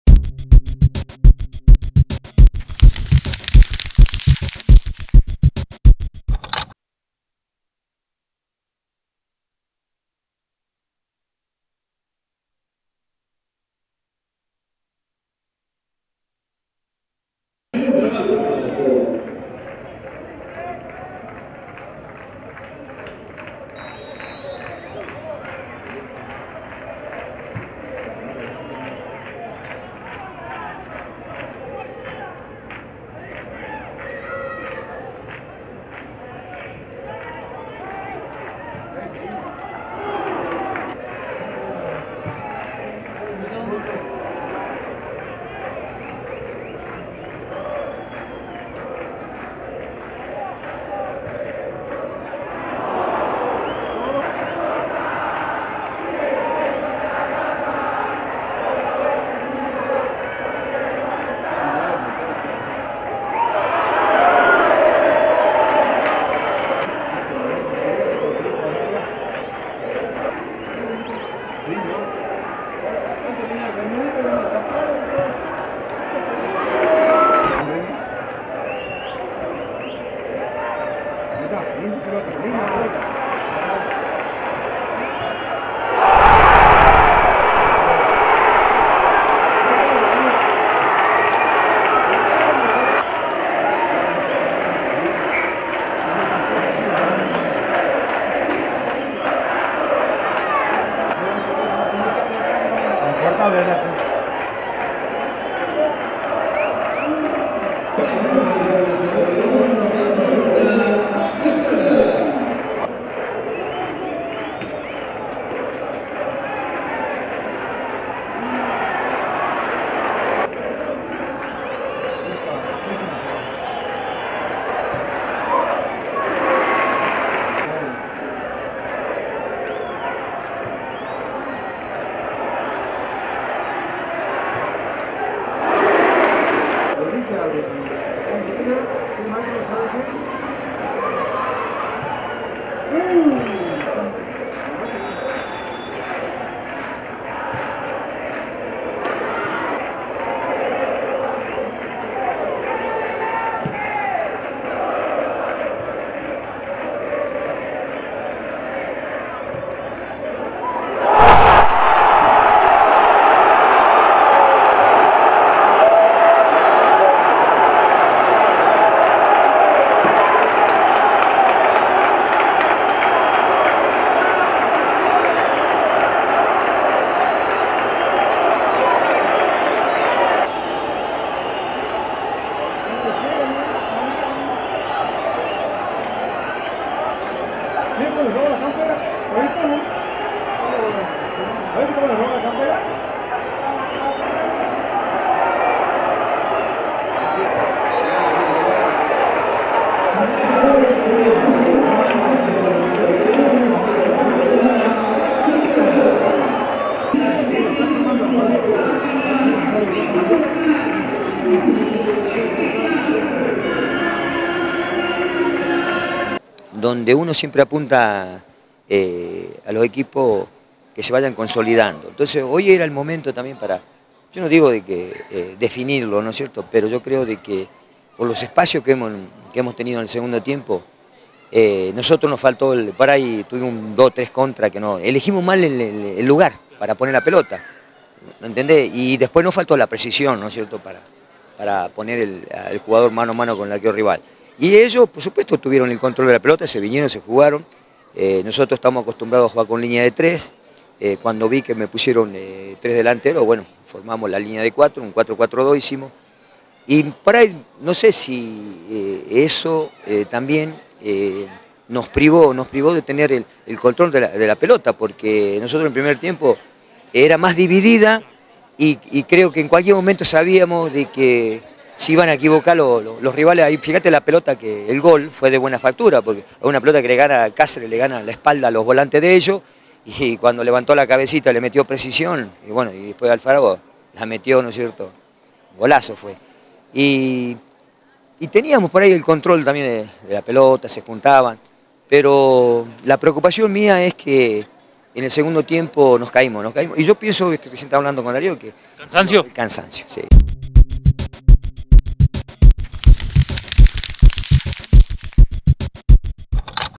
Central Norte 1 - Mitre 1
Imágenes del partido jugado el 5 de diciembre de 2014